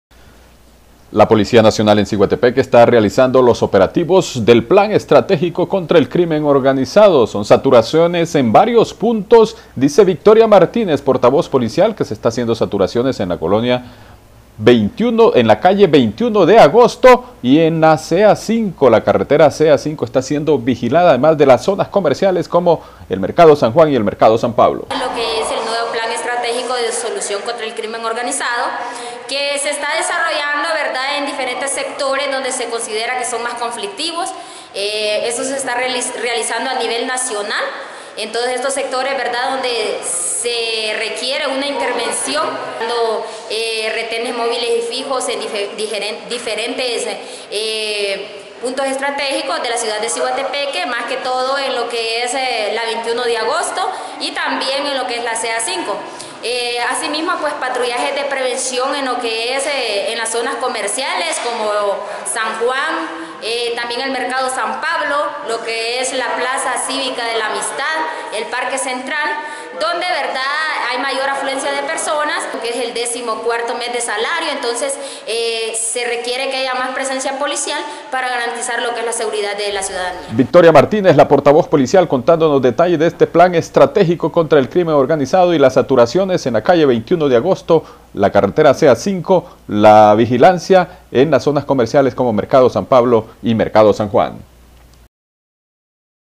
Saturaciones policiales en distintos puntos, buscando delincuentes que perjudican la tranquilidad
La portavoz policial Victoria Martinez dice que el plan estratégico contra el crimen organizado ha permitido la captura de varias personas en todos los operativos que se han realizado.